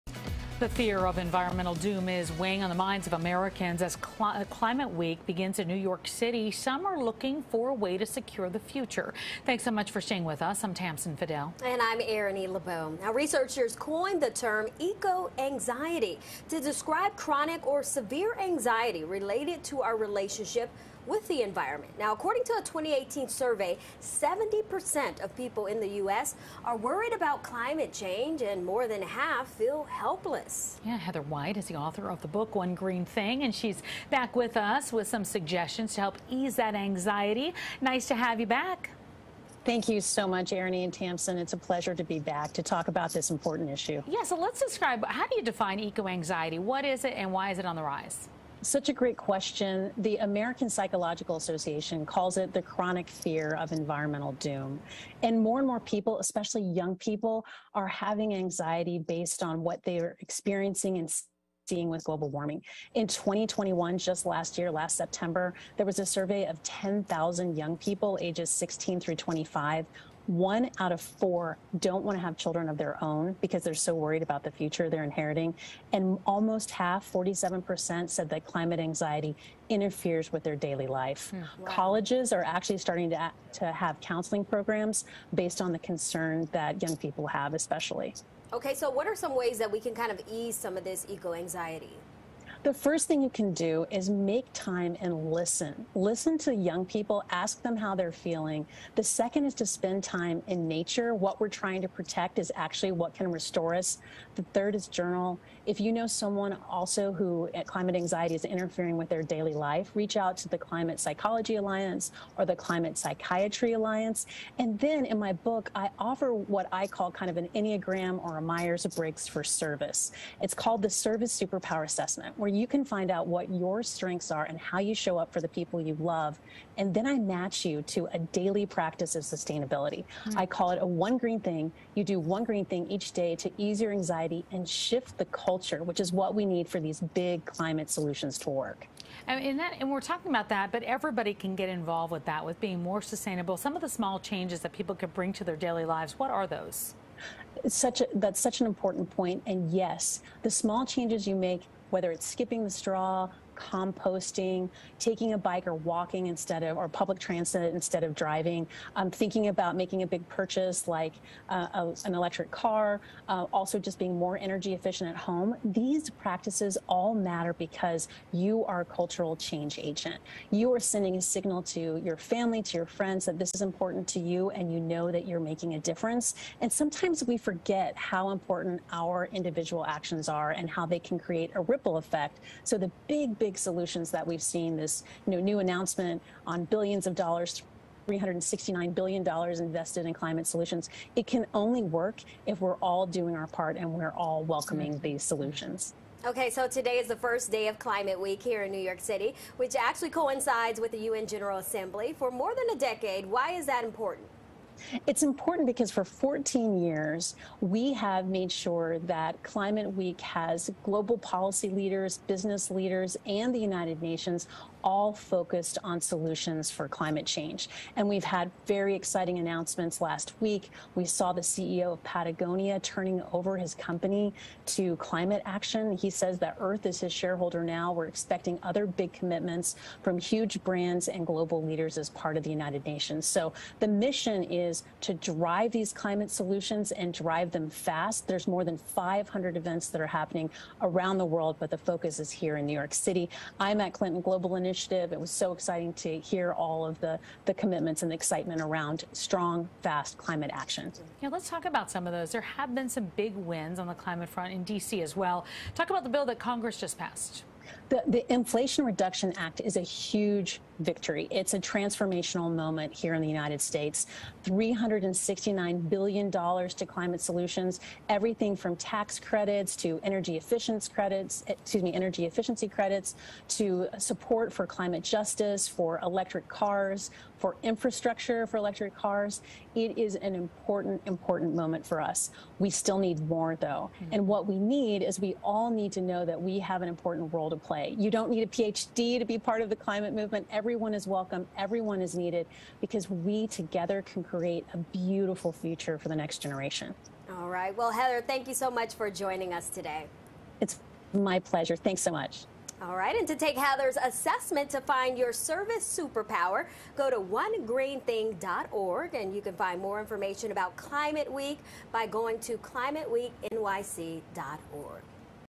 Dealing with eco-anxiety
How to manage feelings of anxiety about the environment